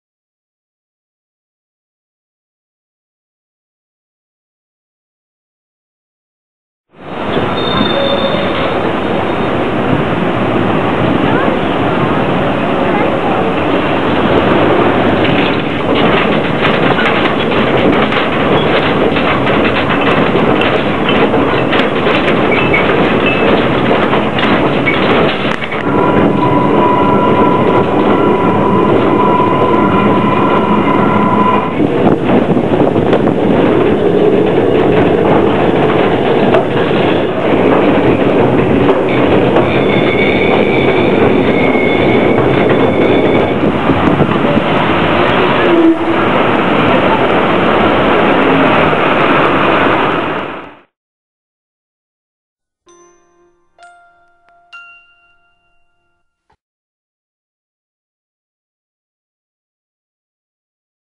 Chile - Valparaiso Ascensores
Valparaiso on the pacific coast in Chile - the city still has trolley buses and also the famous ascensores leading up to the hillside neighbourhoods.  There are 15 of them built between 1883 and 1916.